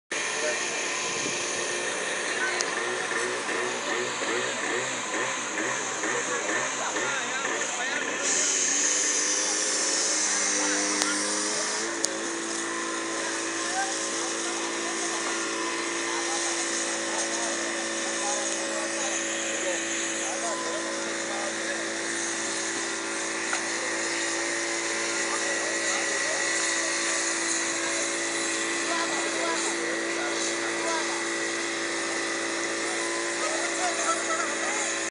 Hulhumale boat fire1 - manadhoolive.com
Amin Construction goods vessel engine room fire at Hulhumale port